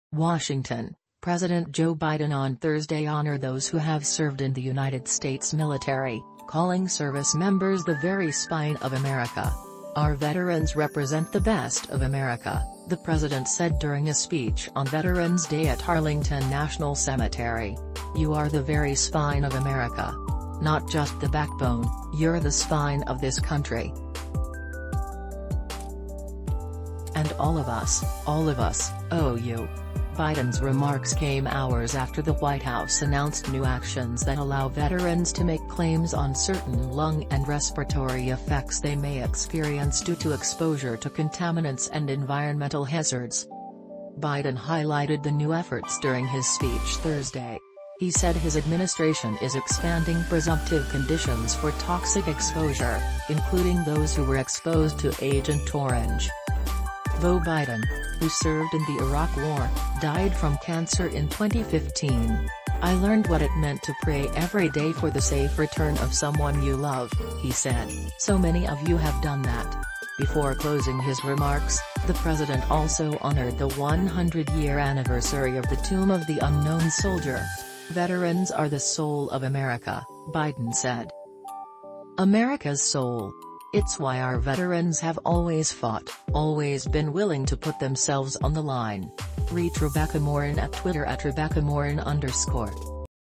'The best of America' Biden honors military members at Veterans Day
WASHINGTON – President Joe Biden on Thursday honored those who have served in the United States military, calling service members the “very spine of America.”"Our veterans represent the best of America,” the president said during a speech on Veterans Day at Arlington National Cemetery. “You are the very spine of America. Not just the backbone, you're the spine of this country. And all of us — all of us — owe you."Biden’s remarks came hours after the White House announced new actions that allow veterans to make claims on certain lung and respiratory effects they may experience due to exposure to contaminants and environmental hazards. Biden highlighted the new efforts during his speech Thursday. He said his administration is expanding presumptive conditions for toxic exposure, including those who were exposed to Agent Orange. The president also noted some service members endure physical wounds, but many also suffer from “unseen psychological wounds of war.”The president also honored several veterans who died in recent weeks, including former Secretary of State Colin Powell, former Georgia Secretary of State Max Cleland and former Army chief of staff Ray Odierno, as well as honoring his late son, Beau. Beau Biden, who served in the Iraq war, died from cancer in 2015.“I learned what it meant to pray every day for the safe return of someone you love,” he said. “So many of you have done that.”Before closing his remarks, the president also honored the 100 year anniversary of the Tomb of the Unknown Soldier.“Veterans are the soul of America,” Biden said. "America's soul. It's why our veterans have always fought, always been willing to put themselves on the line.”Reach Rebecca Morin at Twitter @RebeccaMorin_